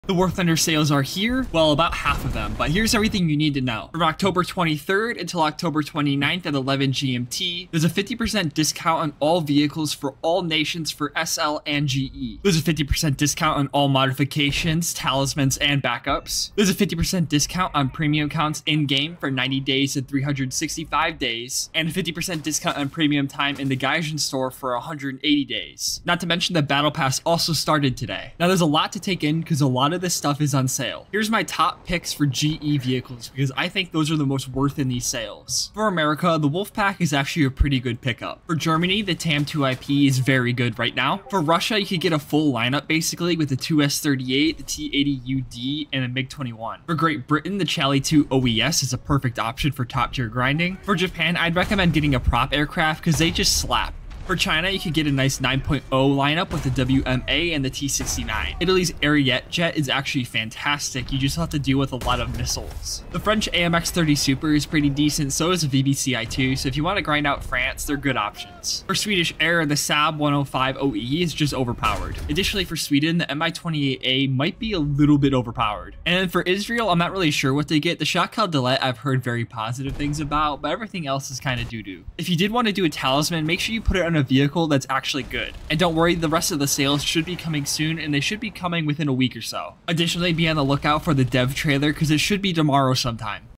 WARTHUNDER SALES ARE HERE (Part 1)
WarThunder's first part of sales have started today. Here are my top picks of what you should get!!!

Discord.gg/nbla